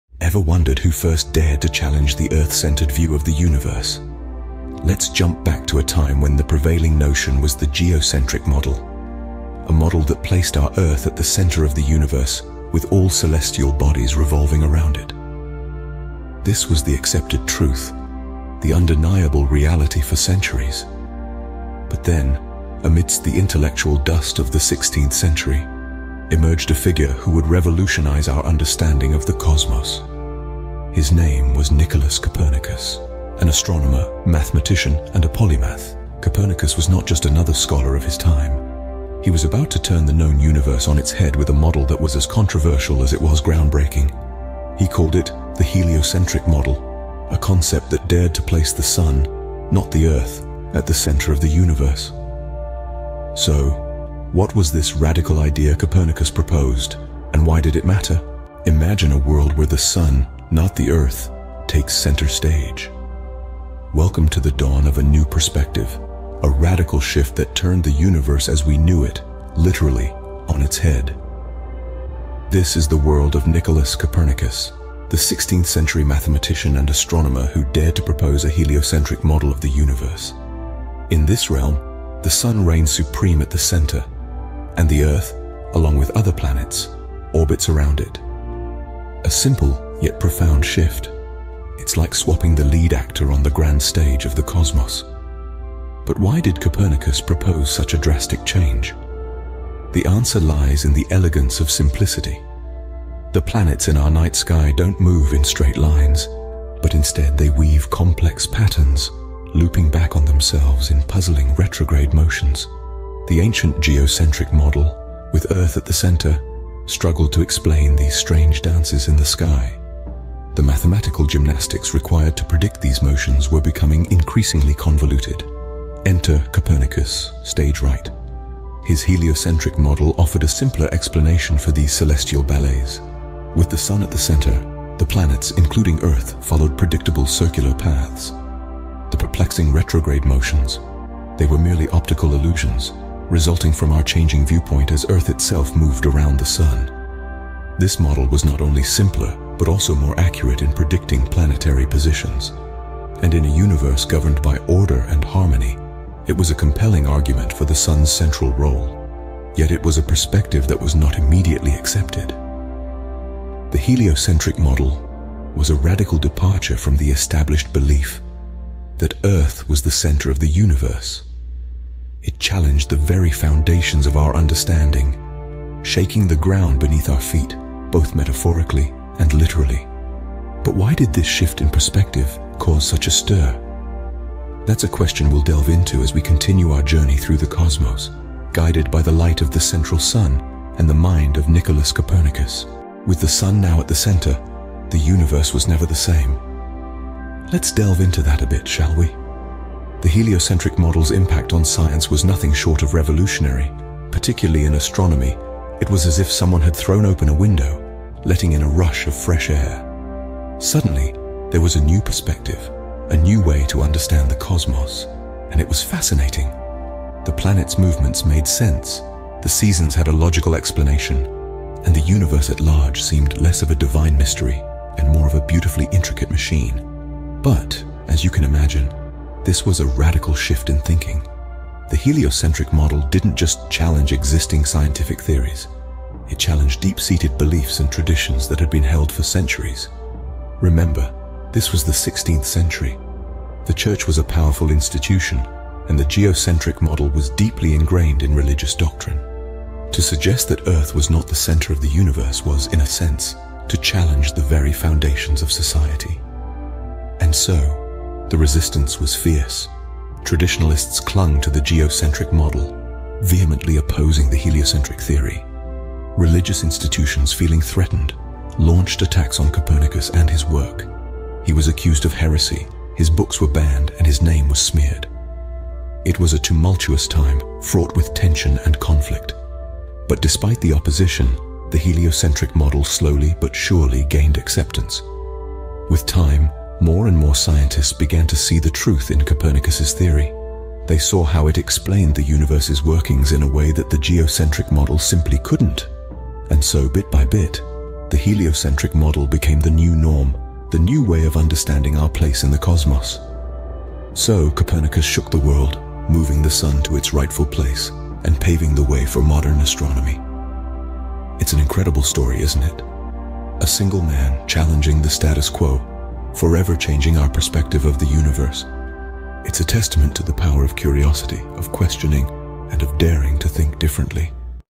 Unraveling the Universe: Copernicus' Heliocentric Model
Embark on a cosmic journey through the revolutionary discoveries of Nicolaus Copernicus as we unravel the mysteries of the universe! Join us in exploring the groundbreaking heliocentric model, which challenged the prevailing geocentric view and forever changed our understanding of the cosmos.

Discover Copernicus' bold insights, astronomical observations, and the profound impact of his heliocentric theory on scientific thought and exploration. 🌍🔭

🔍 Explored Topics:

Nicolaus Copernicus: The Life and Revolutionary Ideas of the Renaissance Astronomer 🌠📚
Heliocentric Model: Understanding the Sun-Centered Universe and its Implications 🌞🌌
Astronomical Observations: Copernicus' Studies of Planetary Motion and Celestial Phenomena 🔭✨
Legacy and Influence: Copernicus' Enduring Impact on Science, Philosophy, and Exploration 🚀🌟
Join us as we unravel the universe through Copernicus' heliocentric model, shedding light on the wonders of our celestial realm. Copernicus HeliocentricModel UniverseExploration 🌟🔭

OUTLINE:
00:00 The Universe in Question
00:58 The Dawn of a New Perspective
03:12 The Impact and the Resistance